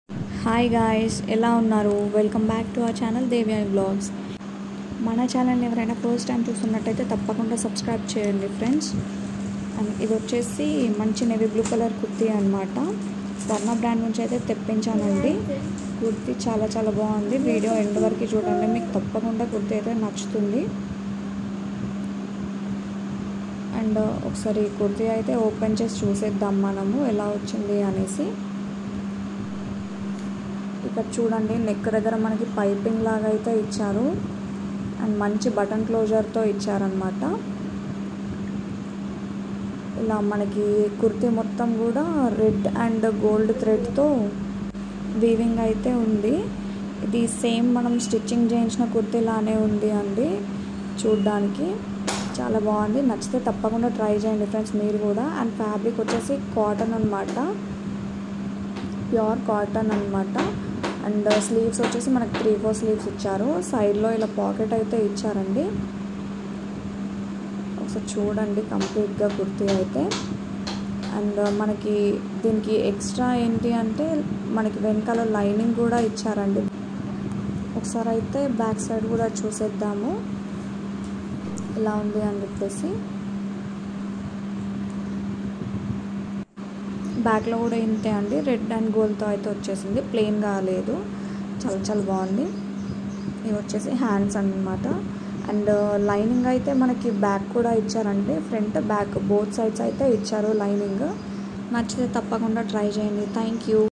Nen pettina cost ki ah quality chusi shock ayya😱Ajio lo kurti#honestreview#ajio#ajiohaul#ytshorts
Ajio best quality kurti at best price❣️ Highly recommended kurti from svarna brand